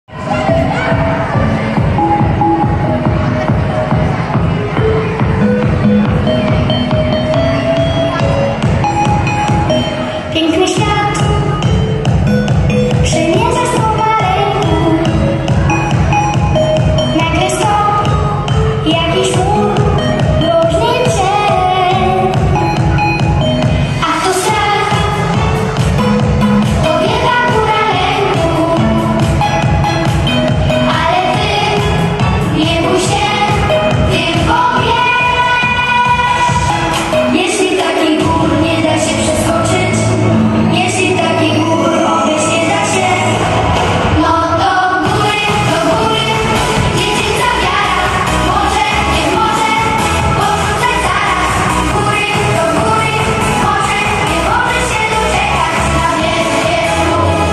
Dzień Papieski w Szkole Podstawowej w Nowych Iganiach
18 października w Szkole Podstawowej w Nowych Iganiach miał miejsce Dzień Papieski.